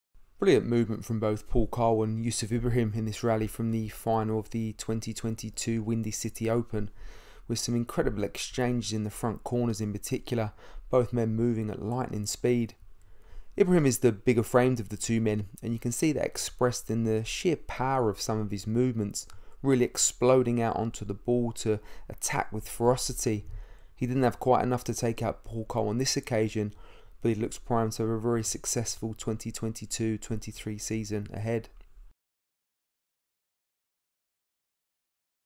Squash Fitness Analysis: Coll vs Ibrahim
Some excellent movement from both Paul Coll and Youssef Ibrahim in this rally, but it's the sheer explosive power of Ibrahim that is perhaps the most eye-catching